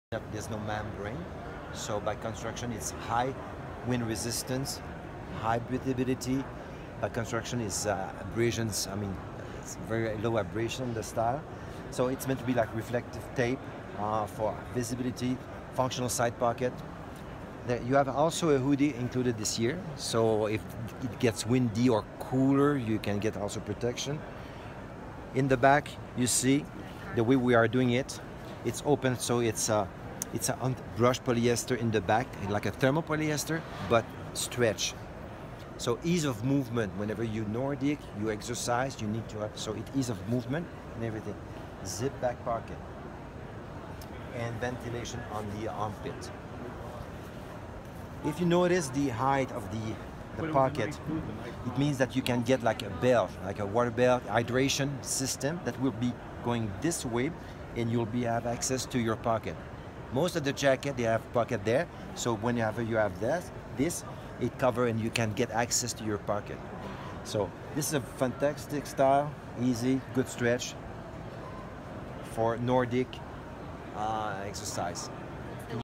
OR/SIA Snow Show 2018 - Louis Garneau
Pierre describing highly functional jacket from Garneau.